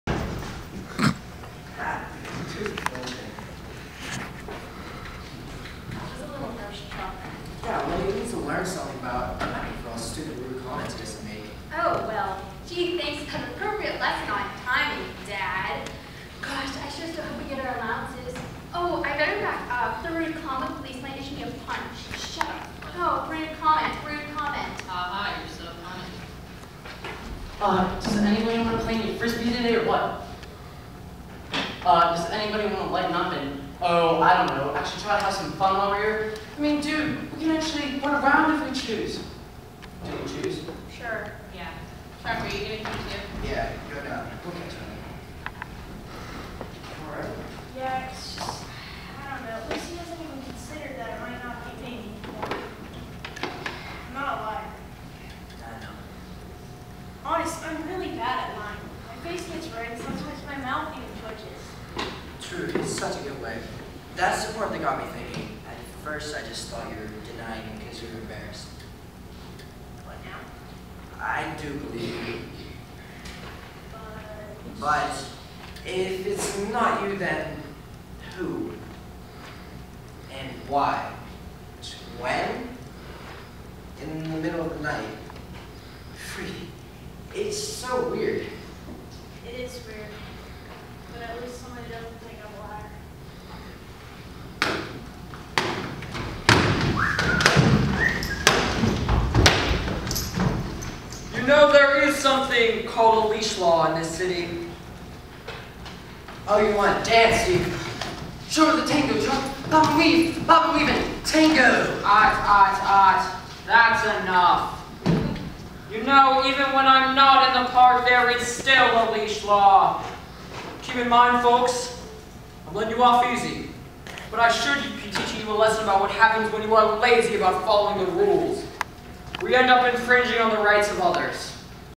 Kate - "Percy" #3
7/10/2014 - CTA Summer Session - Kate Fauss "The Ring of Truth" by Robin Gibson